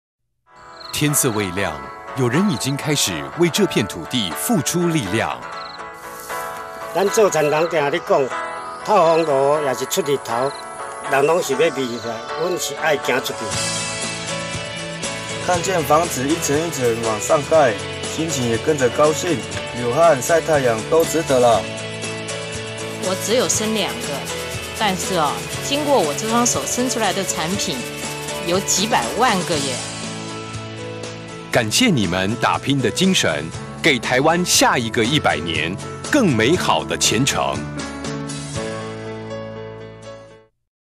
中華民國建國一百年廣告：基層勞工篇(1)
在農田中、工地裡、工廠內
台灣每個角落
都有一群生活簡單，工作卻不簡單的人
為台灣今日的成就，默默奉獻一己之力
感謝您們打拼的精神
給臺灣下一個一百年更美好的前程

中華民國 精彩一百